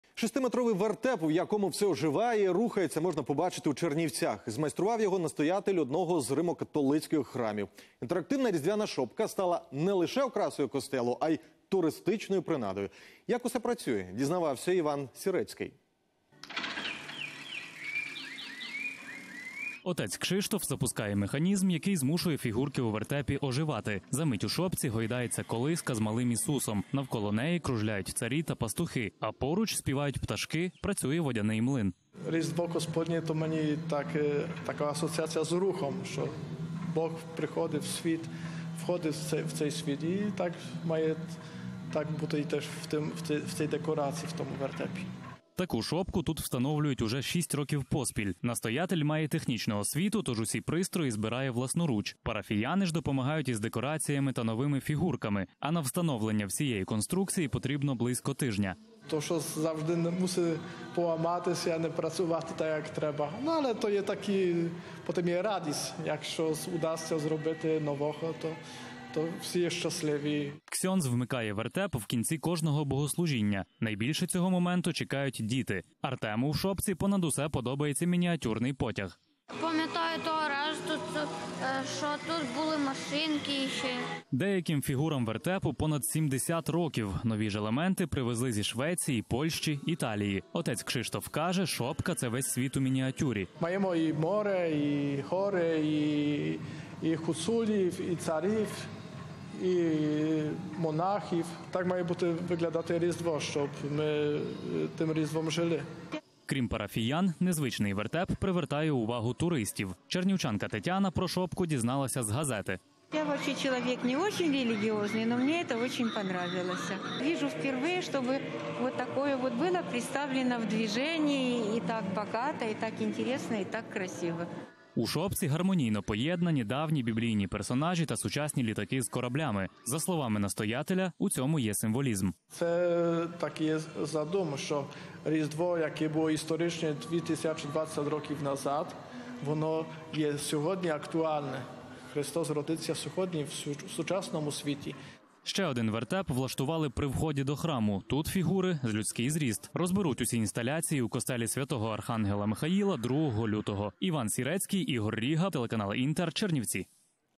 Настоятель храму у Чернівцях змайстрував інтерактивний Різдвяний вертеп
Вертеп, у якому все оживає і рухається став не лише окрасою костелу, а й туристичною принадою